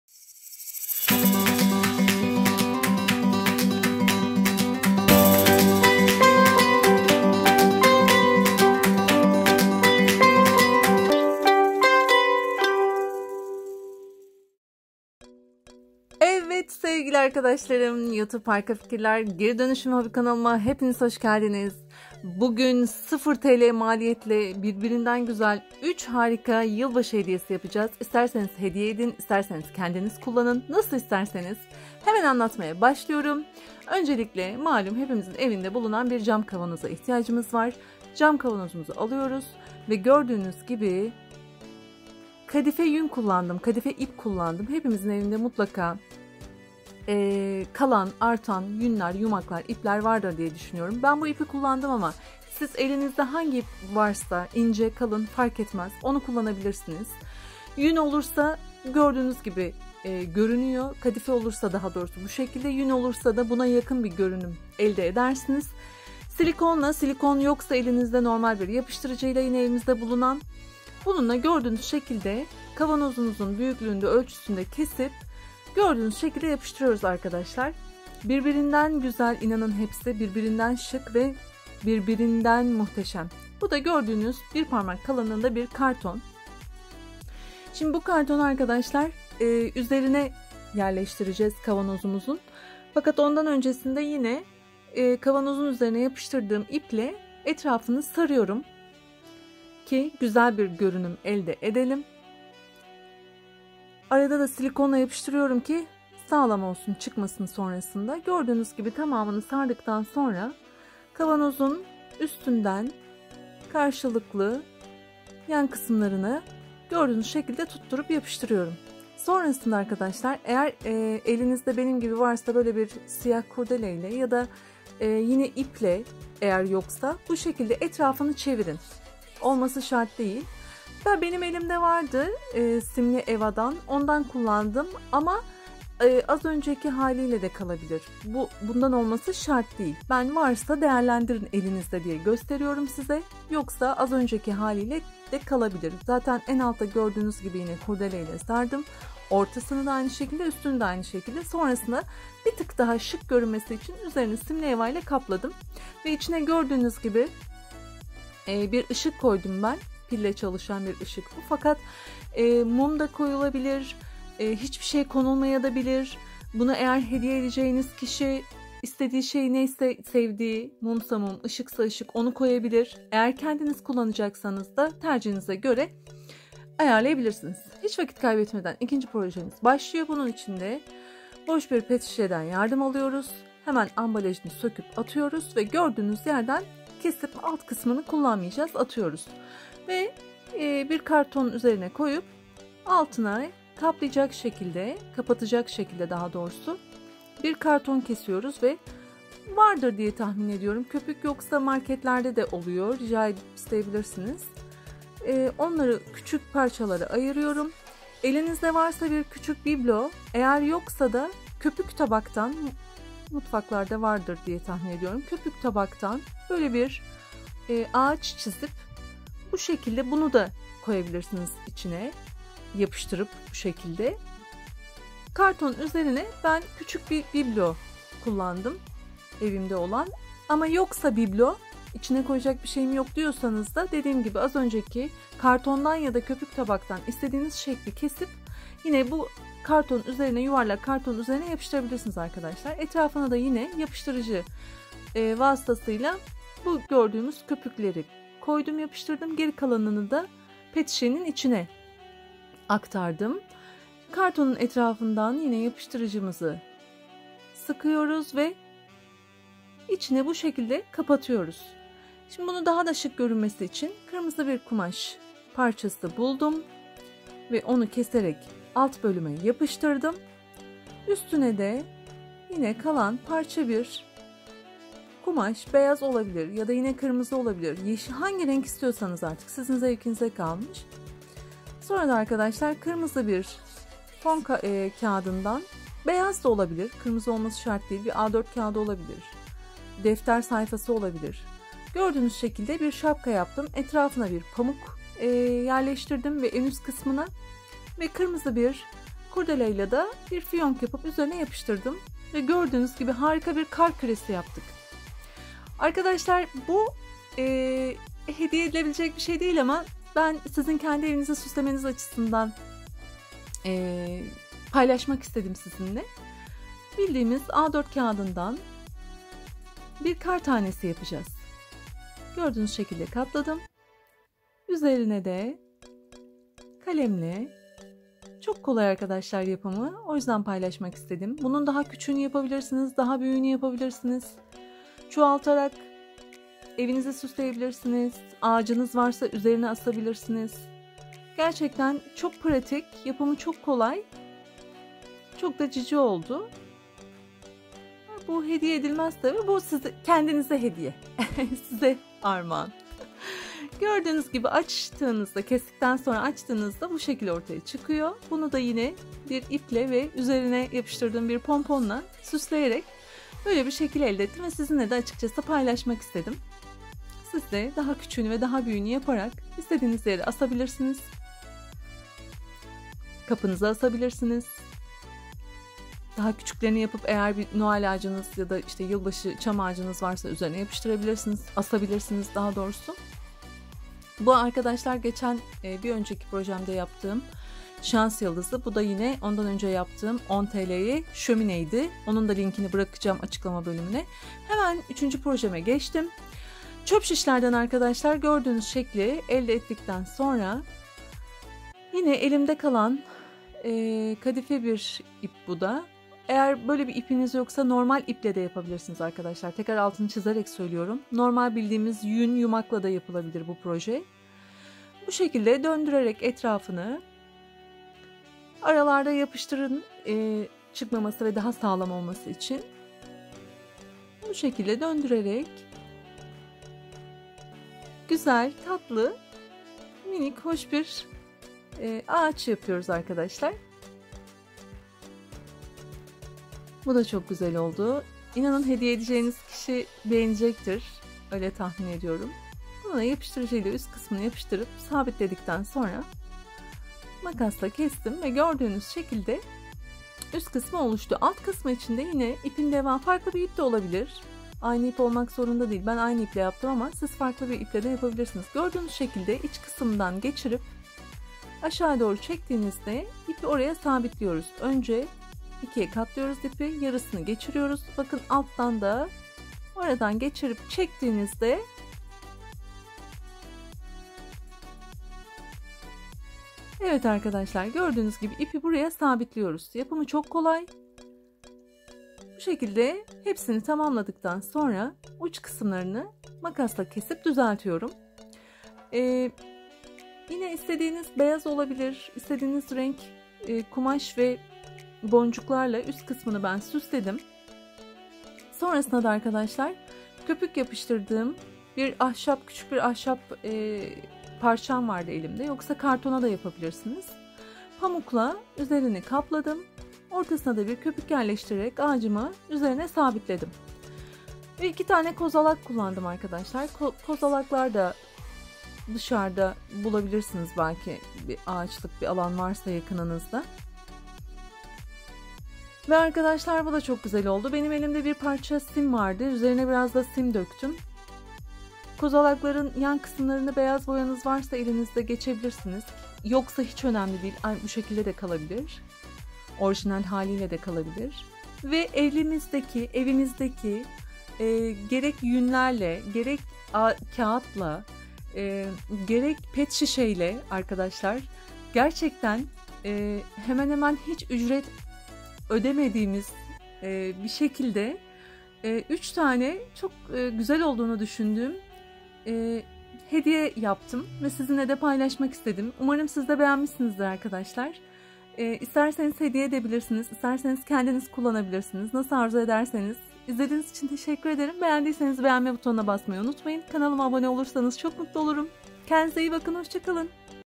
🎁"0" TL' ye 4 HARİKA HEDİYE / YILBAŞI HEDİYELERİ KENDİN YAP / Christmas Decorations
Merhaba arkadaşlar ekonomik yeni yıl hediyeleri yapımı ile siz de sevdiklerinize  sıfır lira maliyetle birbirinden güzel 4 ayrı hediye yapabilirsiniz. Yılbaşı hediyeleri kendin yap.
Geridönüşüm ve hobi kanalımda ev dekorasyonu, dekoratif obje ve aksesuar yapılışı videolarını izleyebilir, görmek istediğiniz projeleri yorumlara yazabilirsiniz.

Keyifli Seyirler.